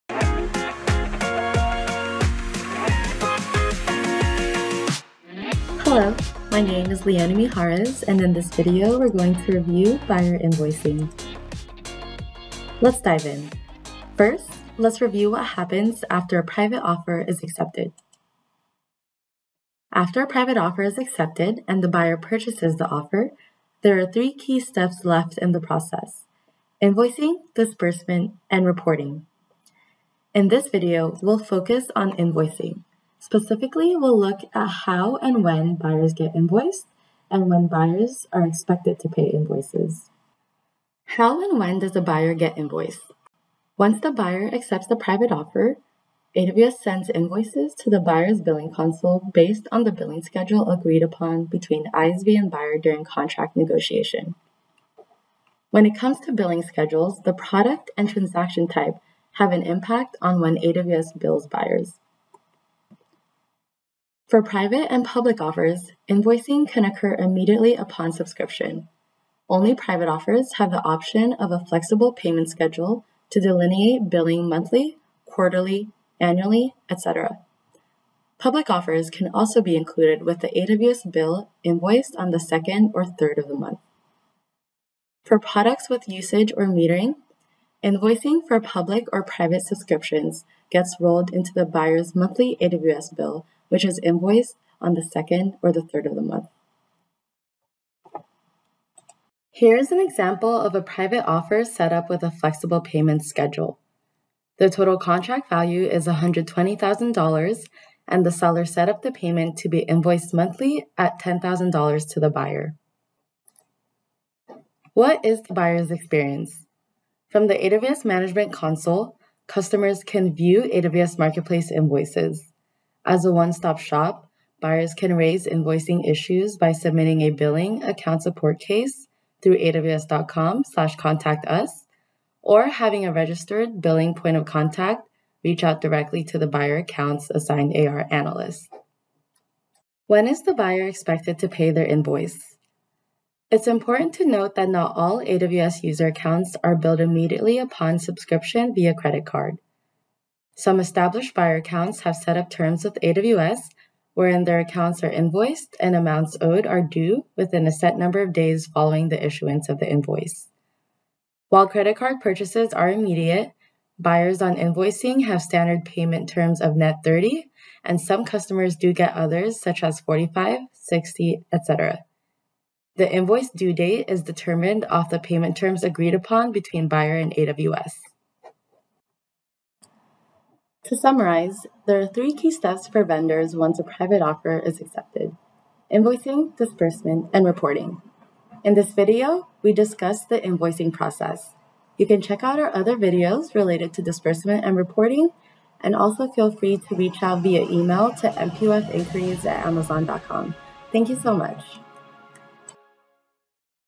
Buyer Invoicing - AWS Marketplace Private Offers
This video covers how a buyer is invoiced for an AWS Marketplace Private Offer. It first covers the three key steps after a private offer is accepted and purchased by a buyer: Invoicing - Disbursement - Reporting. This video dives into billing schedule options and parameters for a buyer.